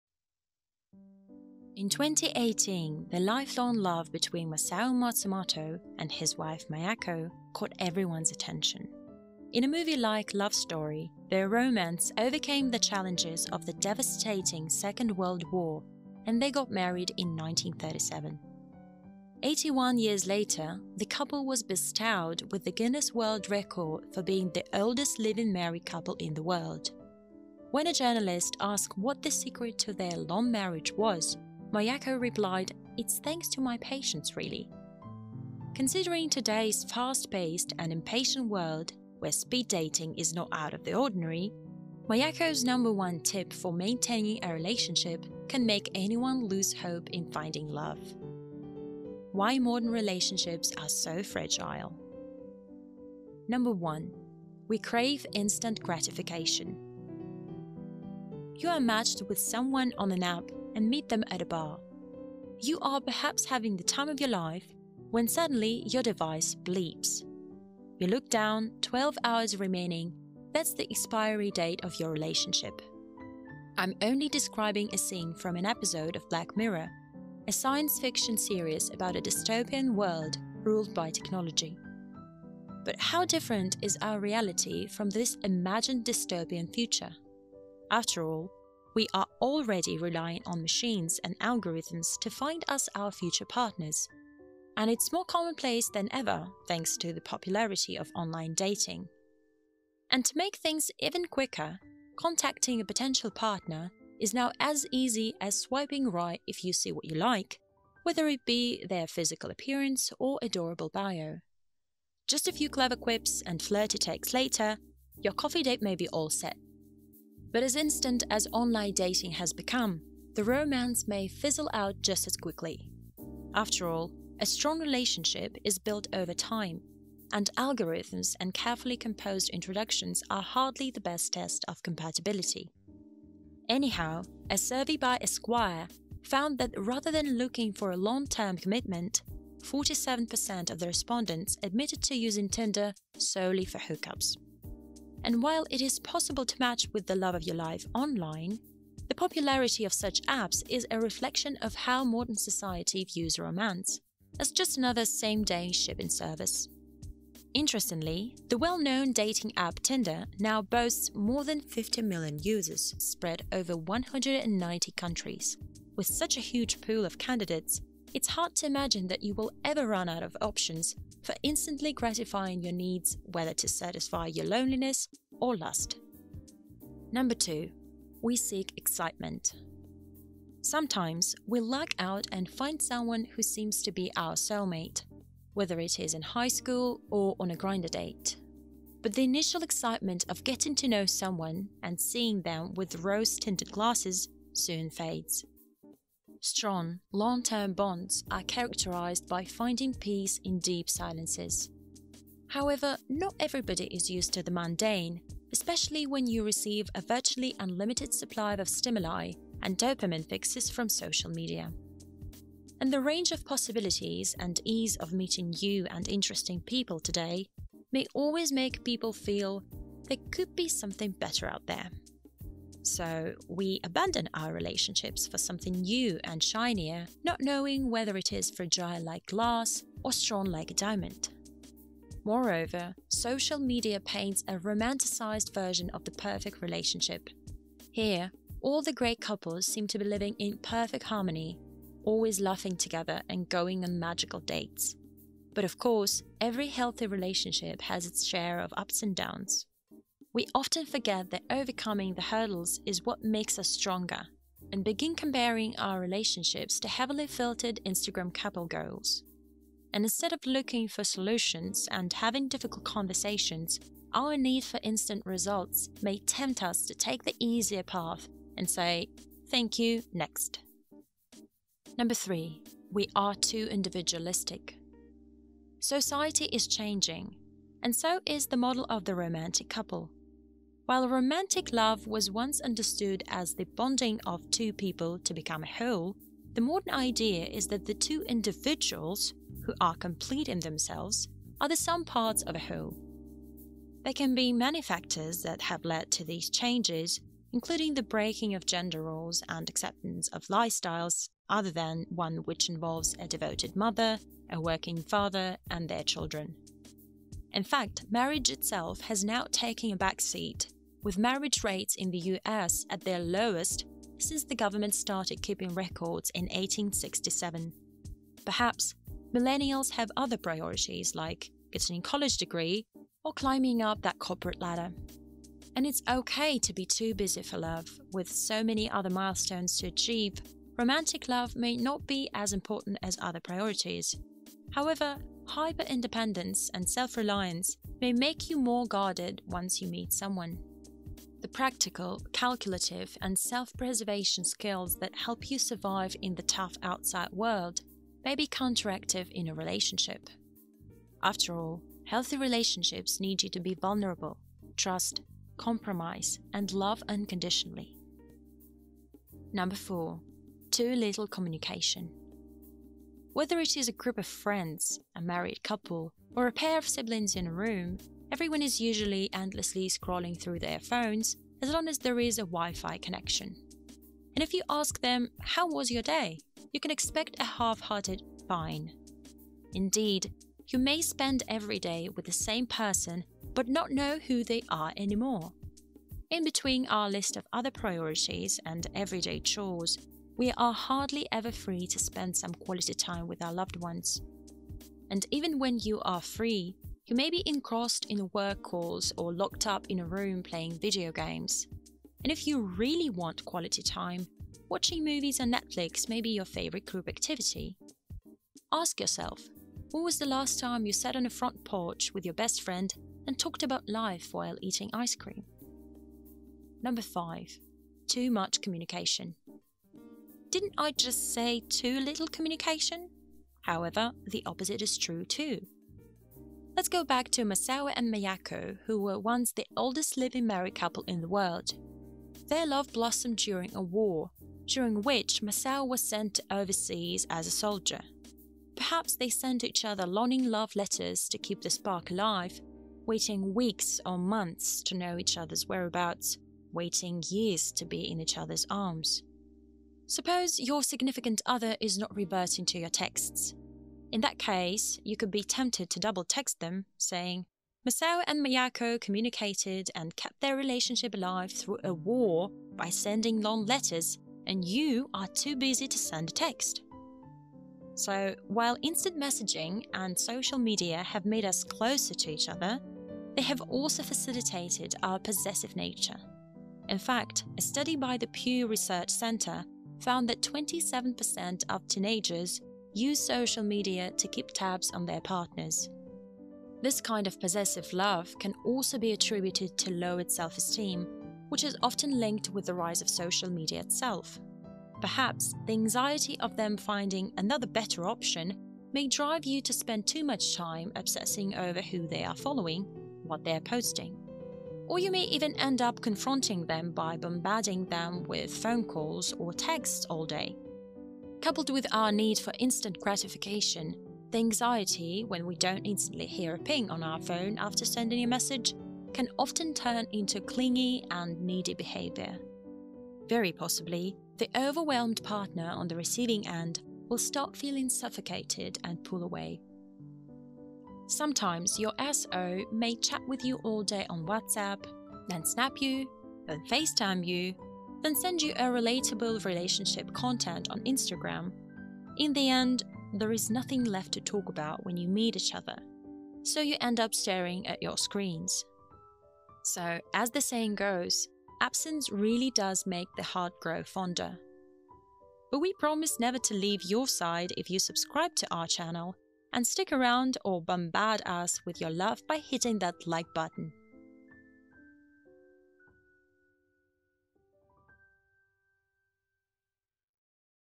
Why Modern Relationships Are Fragile
If you are wondering why modern relationships are so fragile, why they feel like they have an expire date, watch the video to find out more!

In 2018, the lifelong love between Masao Matsumoto and his wife Miyako caught everyone’s attention. In a movie-like love story, their romance overcame the challenges of the devastating Second World War, and they got married in 1937.
Eighty-one years later, the couple was bestowed with the Guinness World Record for being the oldest living married couple in the world. When a journalist asked what the secret to their long marriage was, Miyako replied, “it’s thanks to my patience, really!”
Considering today’s fast-paced and impatient world, where “speed dating” is not out of the ordinary, Miyako’s number one tip for maintaining a relationship can make anyone lose hope in finding love. "

We bet your relationship is different, watch the video to find out what changed!

Chapters
0:00 - Intro
0:52 - We Crave Instant Gratification
2:56 - We Seek Excitement
4:31 - We Are Too Individualistic
5:58 - Too Little Communication
6:56 - Too Much Communication
8:50 - Outro